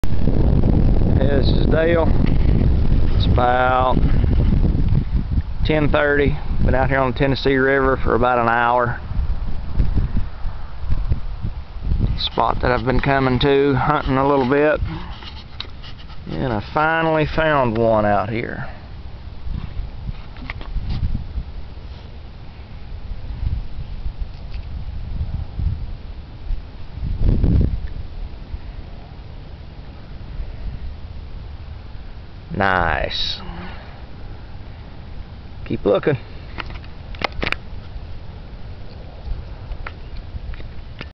Tennessee Arrowhead hunting 3/19/11
East Tennessee arrowhead hunting. My first attempt at making a video, and found a nice one to start with. Hopefully the vids, and points, will get better as I go.